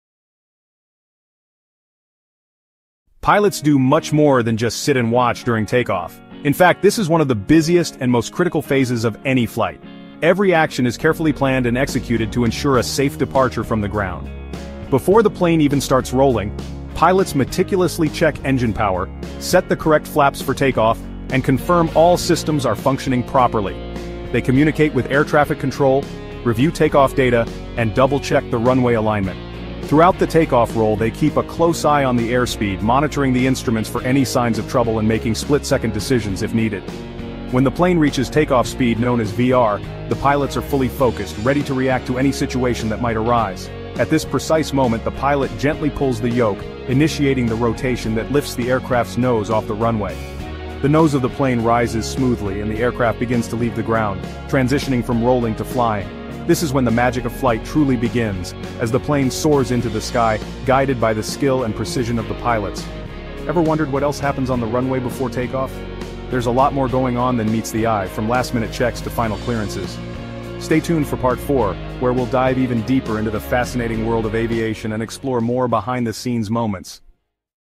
What Pilots Do During Takeoff 🧑‍✈️ | Inside the Cockpit Explained!
Ever wondered what a pilot really does during takeoff?
In this 60-second short by Jig’s Tech Knowledge, go inside the cockpit and discover the key actions pilots take to safely launch a plane into the sky.

🎯 You’ll learn:
✈️ What V1 and VR speeds mean
🧑‍✈️ How pilots control flaps and thrust
⚠️ What they monitor on the dashboard
🎛️ How the yoke is used to rotate the aircraft

This is a must-watch for aviation fans, students, and future pilots!